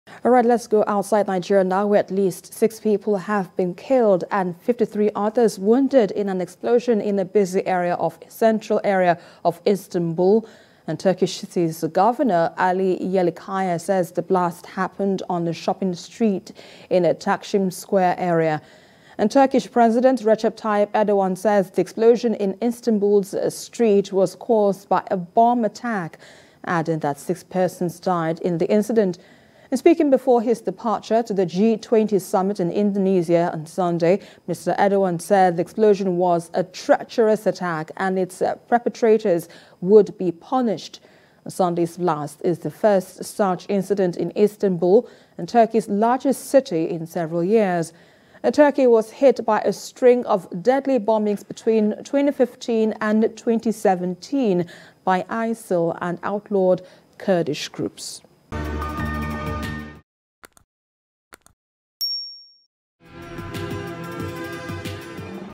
Six K#lled, Dozens Wounded in Istanbul's Istiklal Street in Turkey
Turkish city's governor Ali Yerlikaya says the blast happened on a shopping street in the Taksim Square area.
Turkish President Recep Tayyip Erdogan says the explosion in Istanbul’s Istiklal Street was caused by a “bomb attack,” adding that six people died in the incident.
Speaking before his departure to the G-20 summit in Indonesia on Sunday, Mr Erdogan said the explosion was a “treacherous attack” and its perpetrators would be punished.
Sunday’s blast is the first such incident in Istanbul, Turkey’s largest city, in several years.
Turkey was hit by a string of deadly bombings between 2015 and 2017 by ISIL (ISIS) and outlawed Kurdish groups.